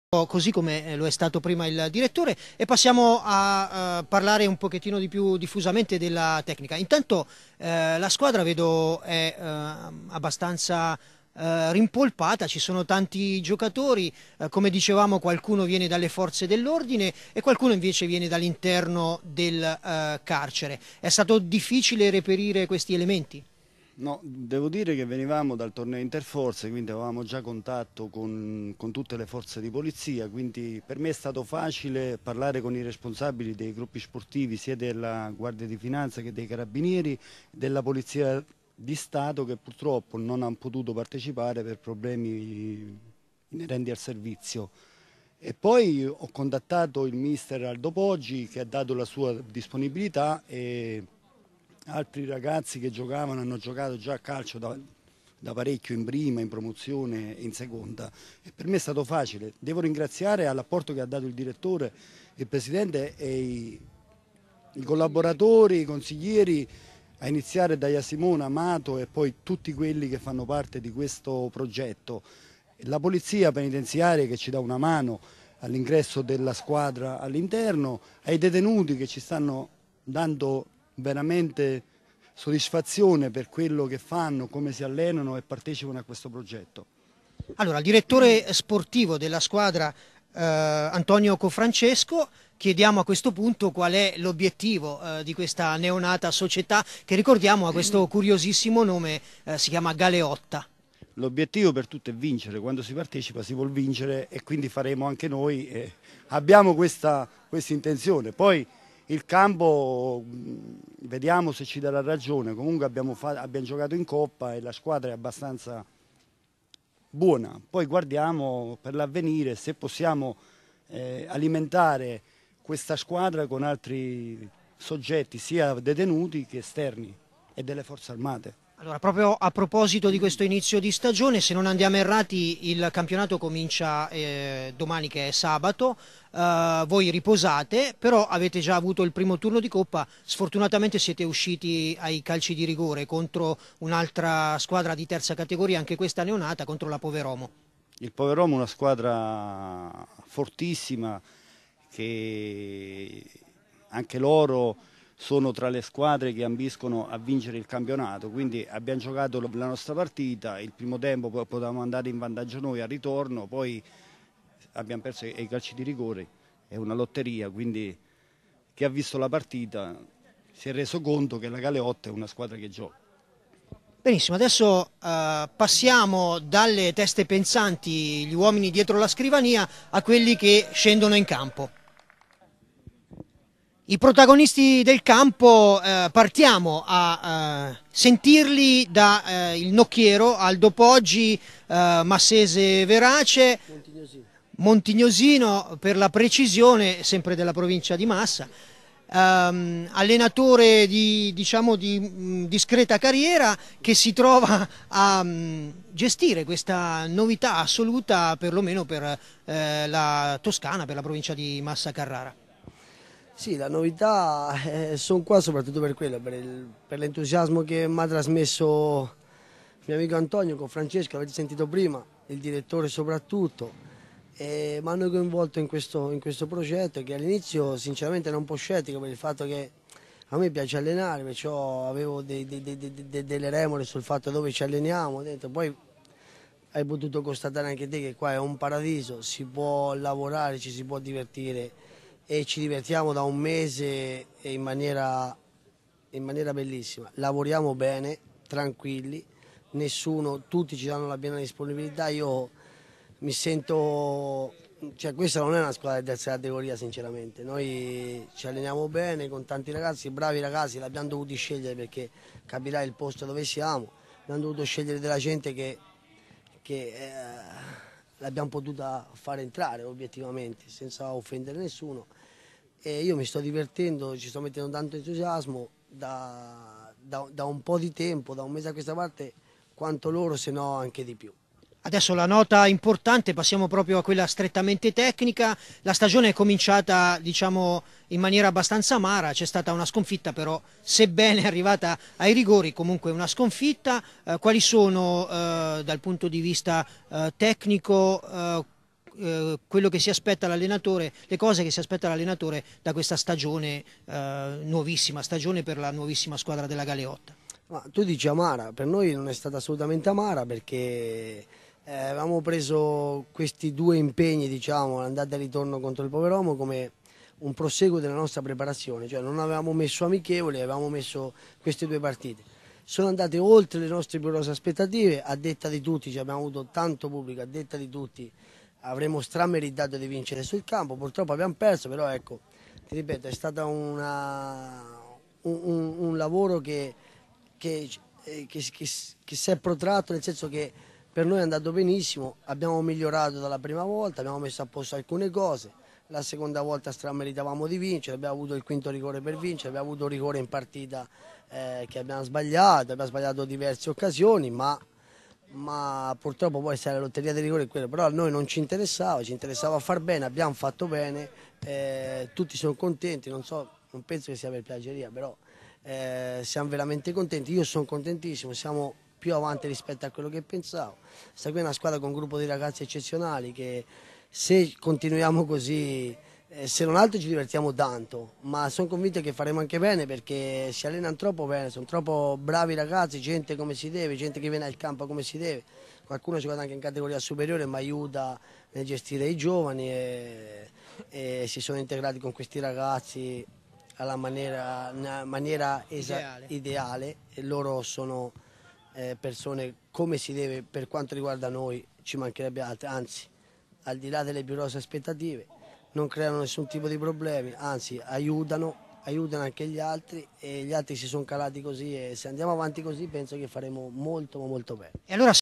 Parte La Galeotta 3^ categoria 2008 / 2009 MS
Prima squadra in Italia composta da detenuti, Carabinieri, Agenti di polizia Penitenziaria, Finanzieri e gente comune